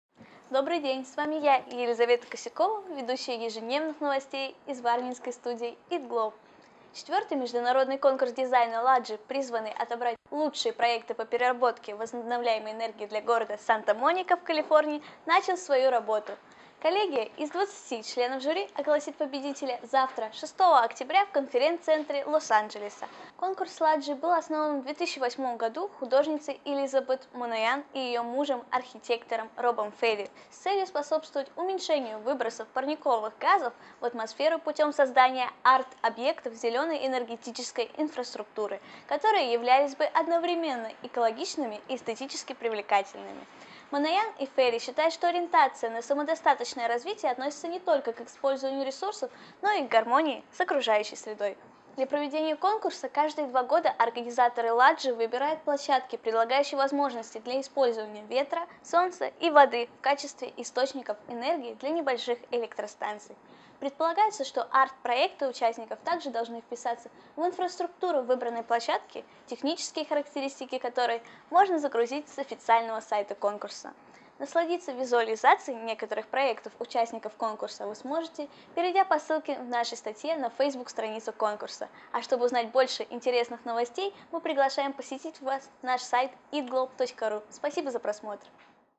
Электростанции как арт-объекты
В мире появляется все больше экоэлектростанций, а задача калифорнийского конкурса это выбор лучших проектов, чьи электростанции не только экологичны, но также и привлекательны.